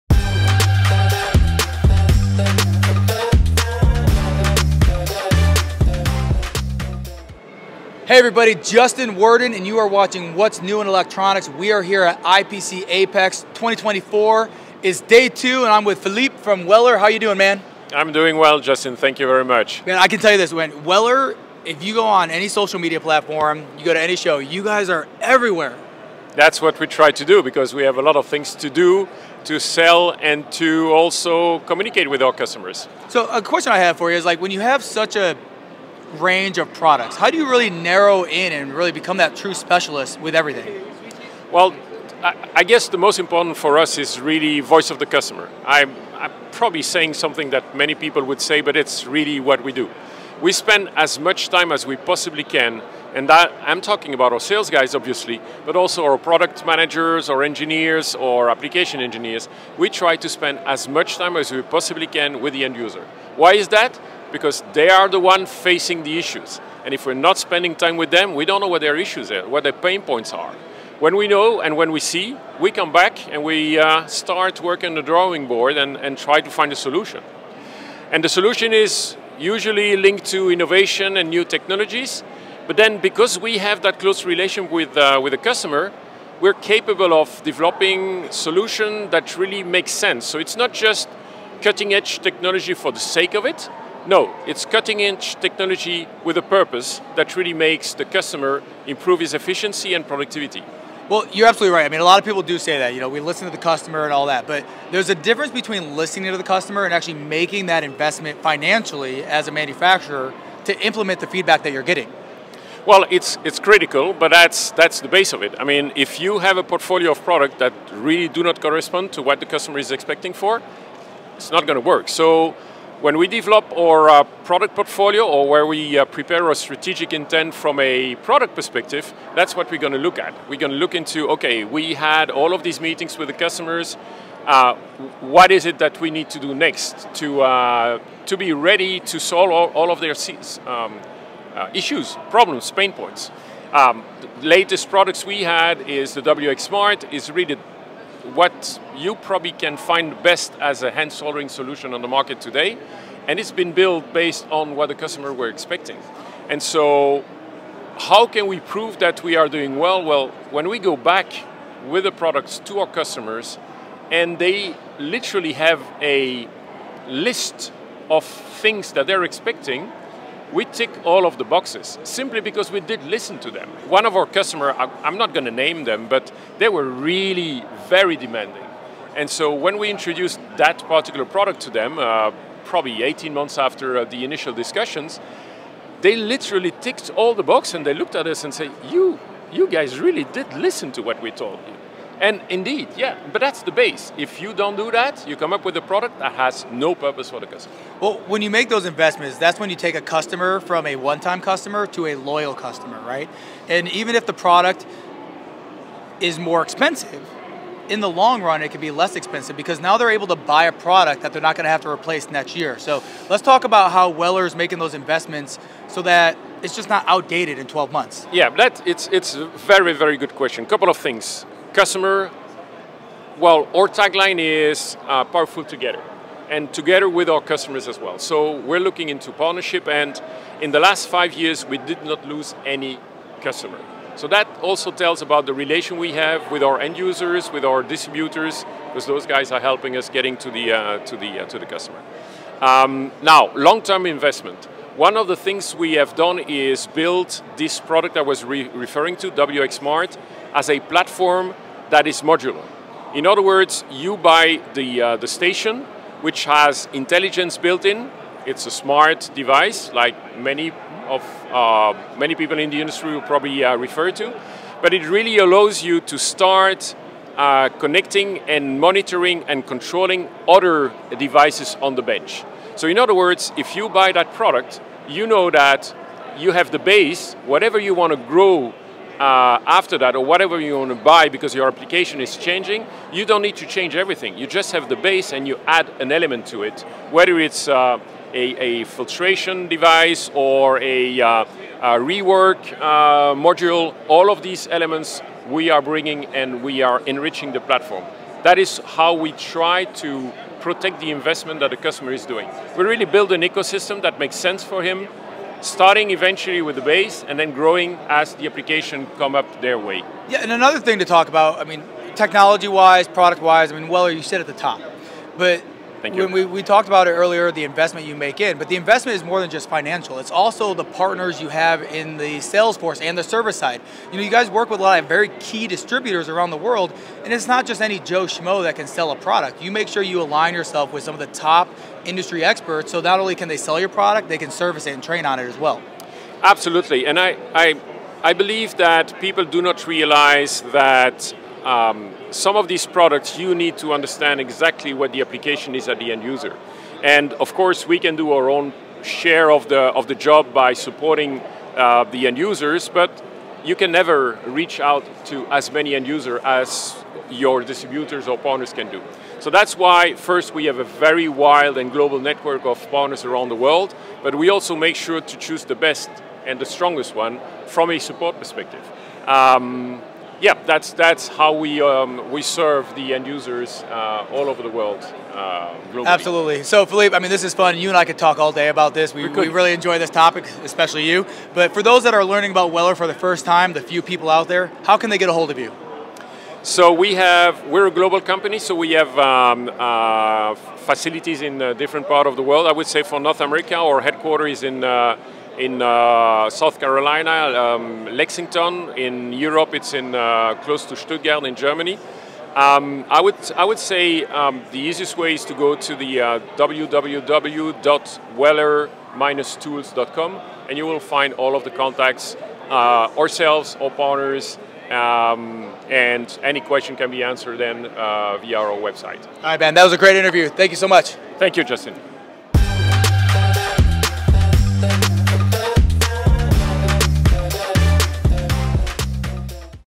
Philippe Biding from Weller at IPC Apex EXPO 2024
Justin Worden speaks to Philippe Biding from Weller at IPC Apex EXPO 2024 in Anaheim, USA